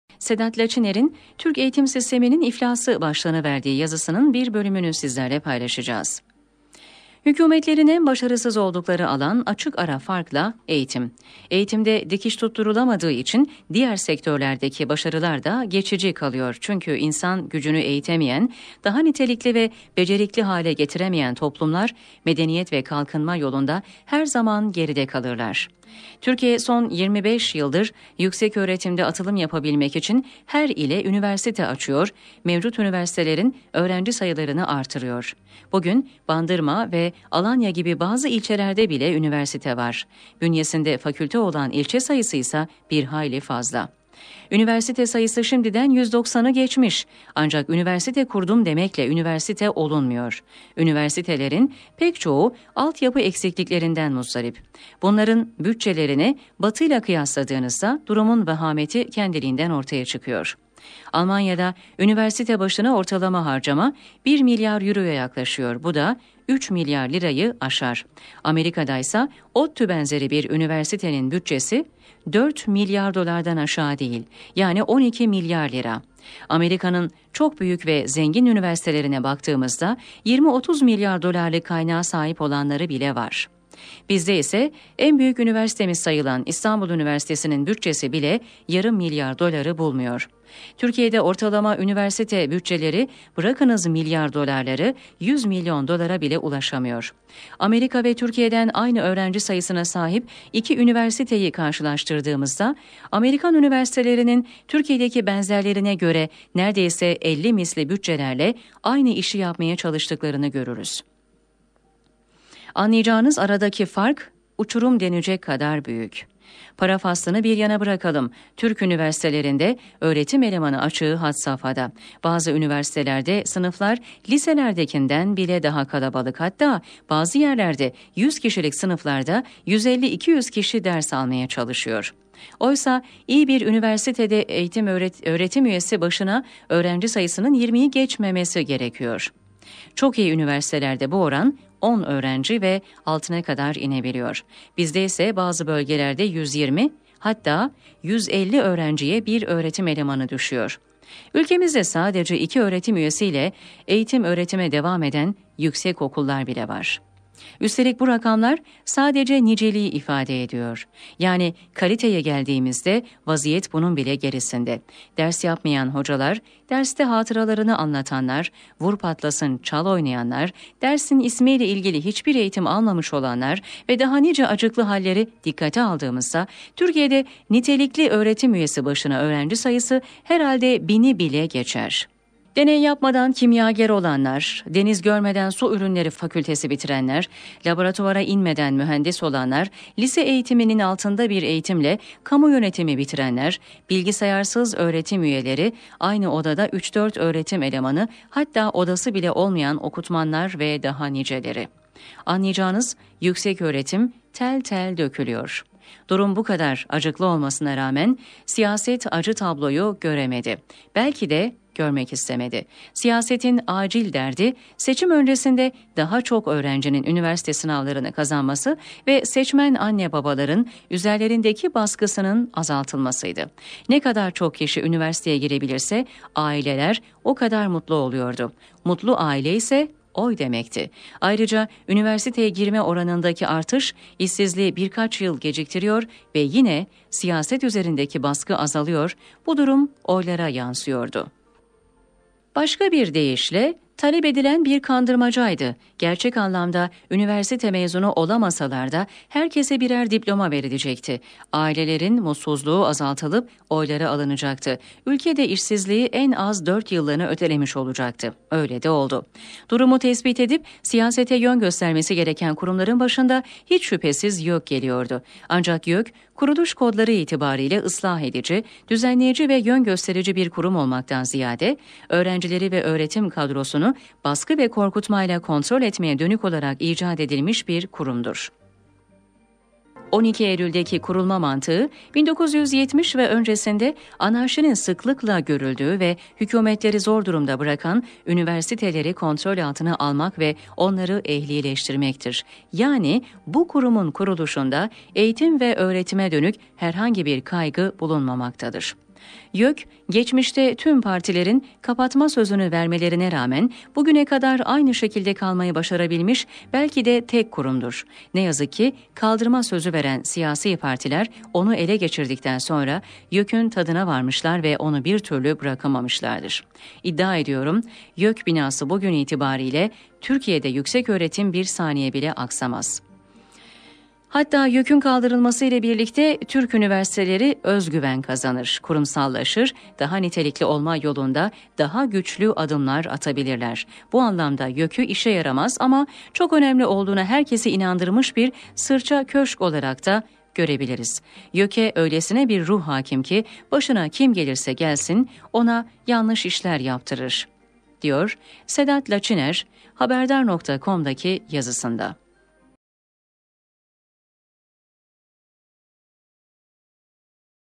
Sedat Laçiner - Türk Eğitim Sisteminin İflası
Hükümetlerin en başarısız oldukları alan, açık ara farkla eğitim. Eğitimde dikiş tutturulamadığı için diğer sektörlerdeki başarılar da geçici kalıyor.

Çünkü insan gücünü eğitemeyen, daha nitelikli ve becerikli hale getiremeyen toplumlar medeniyet ve kalkınma yolunda her zaman geride kalırlar. Türkiye, son 25 yıldır yükseköğretimde atılım yapabilmek için her ile üniversite açıyor, mevcut üniversitelerin öğrenci sayılarını arttırıyor.

Bugün Bandırma ve Alanya gibi bazı ilçelerde bile üniversite var. Bünyesinde fakülte olan ilçe sayısı ise bir hayli fazla. Üniversite sayısı şimdiden 190’ı geçmiş. Ancak 'üniversite kurdum' demekle üniversite olunmuyor.